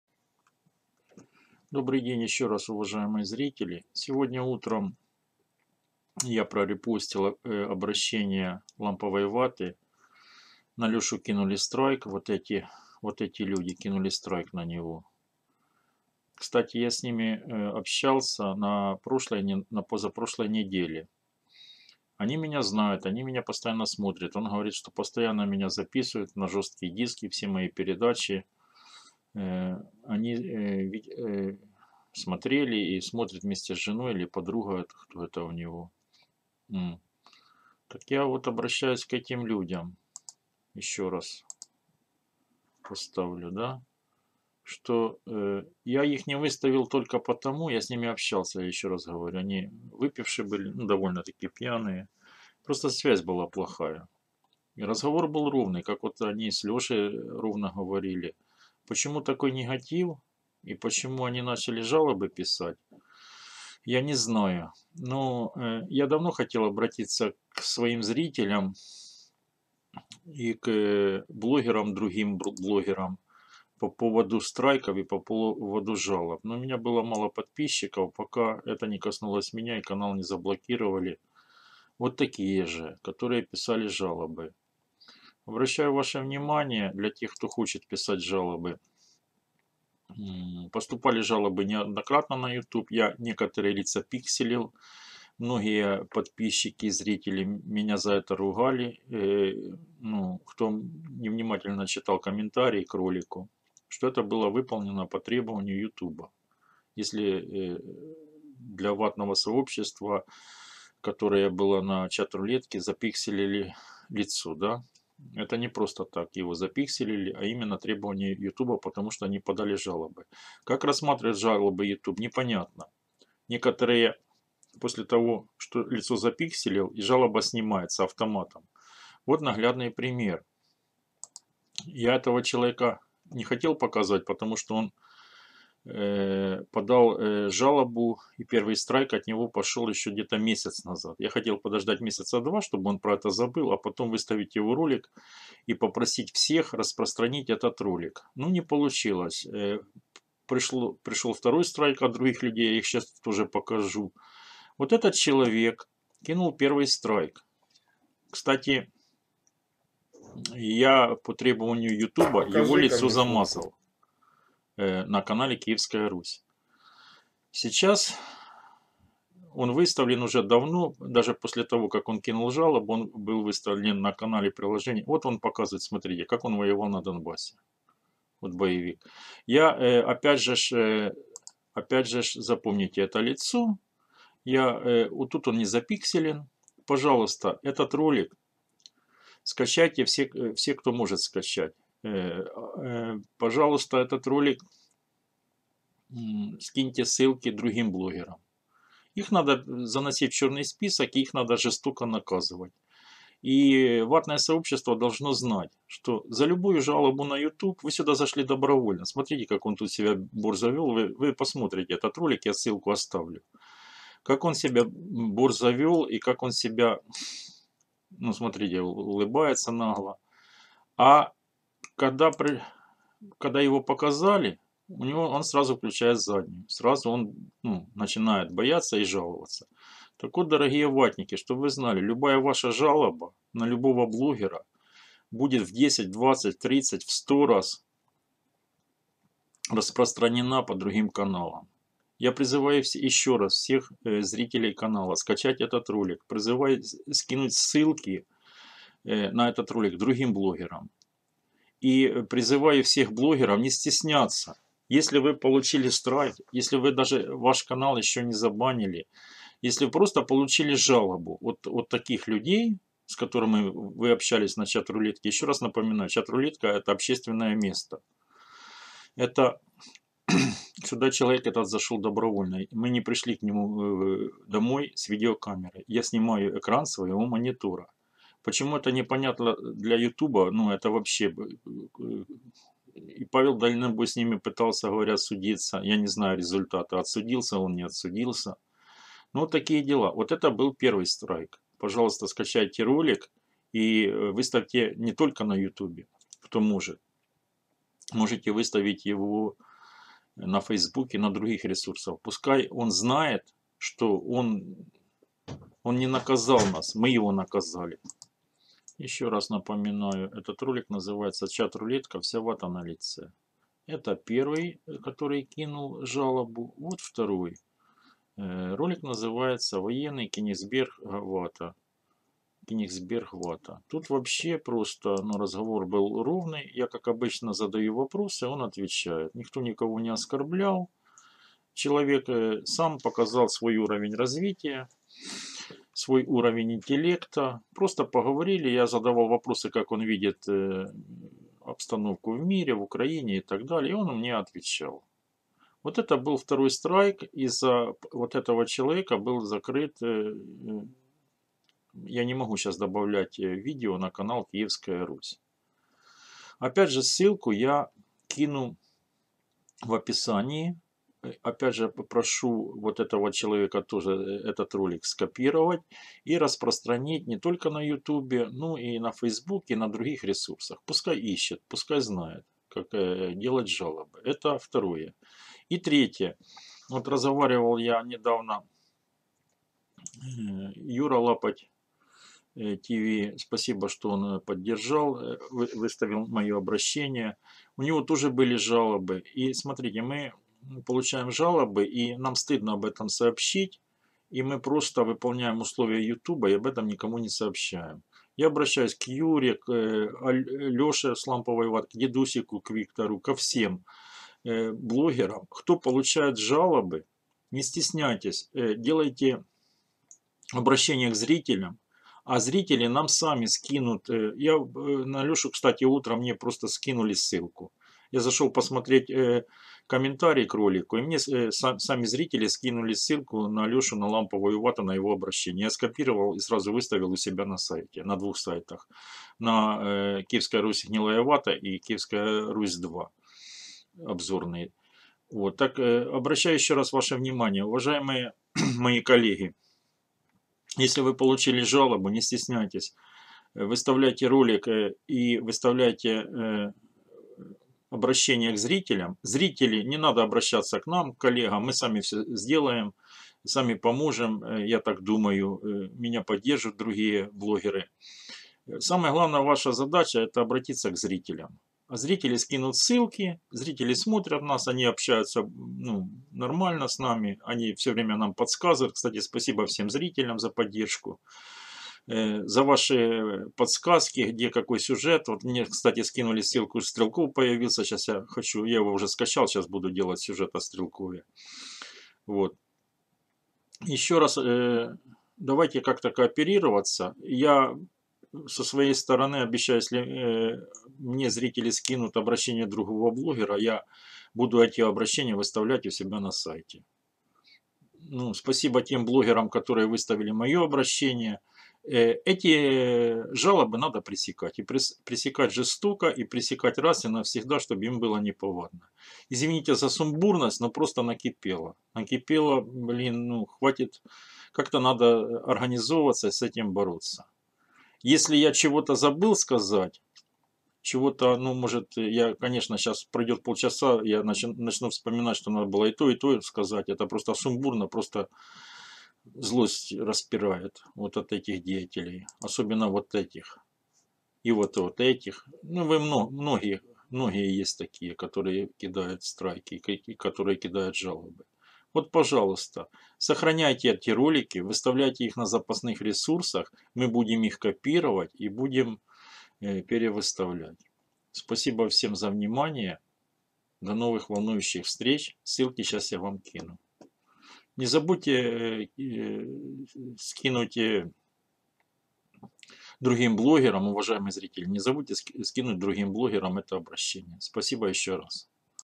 СРОЧНОЕ ОБРАЩЕНИЕ ОТ КИЕВСКОЙ РУСИ
У этих с золотыми зубами комплекс неполноценности. После того как они у Полтавы прославились, они решили всем проукраинским блогерам пакостить. Единственно верный способ борьбы с такими долбодятлами - это множество каналов и репостов.

Категория
Люди и блоги
Лицензия
Стандартная лицензия YouTube